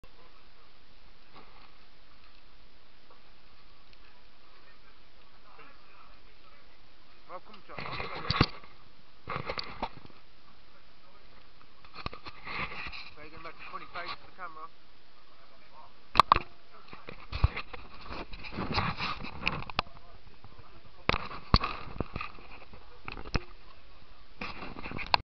the boys 4Q2 at airsoft
pipedawg2007 , trig9 , halfsight , real crab on tour playing someairsoft .